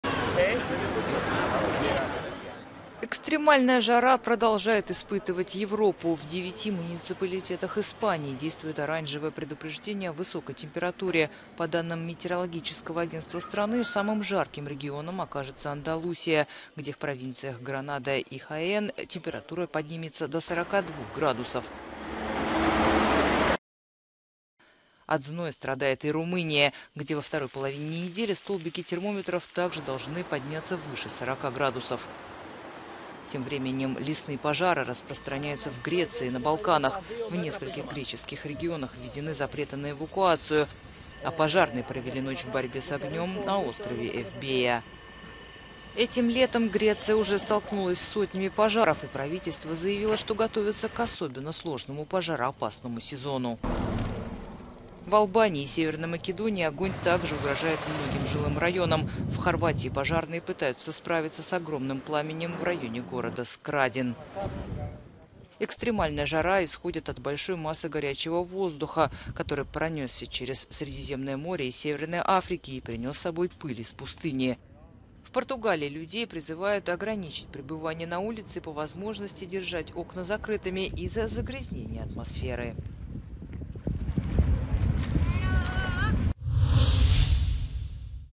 Пыльный горячий воздух из Африки накрыл Европу
Аномальная жара в Европе каждый год — уже не новость. В этот раз высокие температуры в Южную Европу принес с собой горячий воздух из Северной Африки